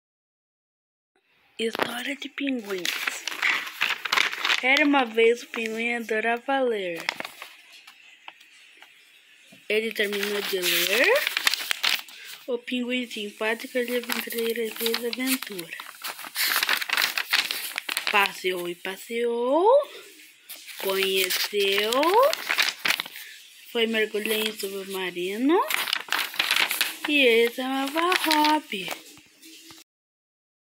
Leitura 01: Histórias de Pinguins - Ulisses Kociuba
Oi, tudo bem criançada? Aqui é o Ulisses e eu tenho 10 anos o livro eu fiz sozinho! Tchau beijos!

Ulisses Kociuba.